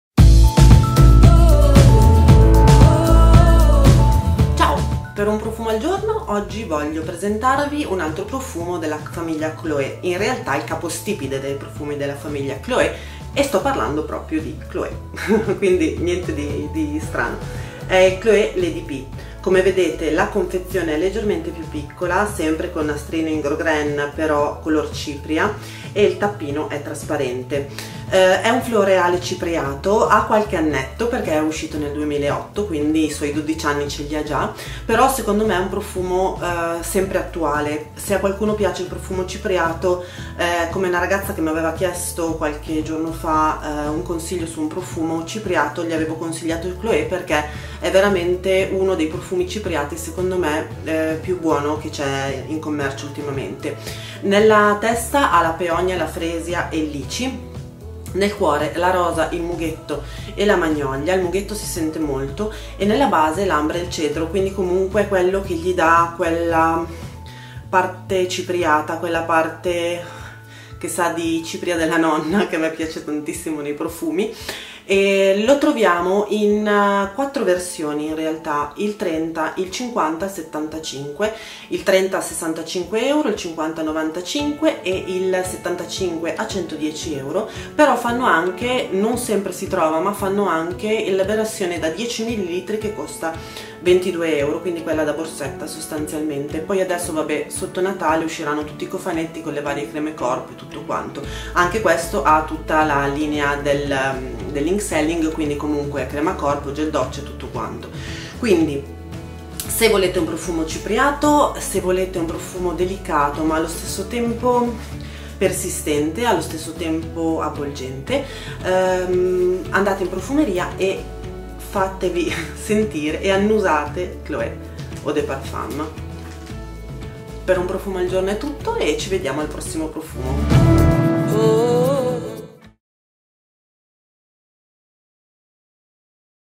Recensione profumi Chloè Eau de Parfum
Ciao, oggi voglio parlarvi di un profumo femminile che mi piace molto, è un floreale cipriato che ricorda tempi antichi, molto fine, delicato ma a suo modo prorompente. Mi piace molto questa fragranza e vi consiglio se non la conoscete di andarla a provare. Ci vediamo al prossimo profumo, un abbraccio
Se volete potete trovarmi anche su
Facebook alla pagina Io e loro

ciaooooooo!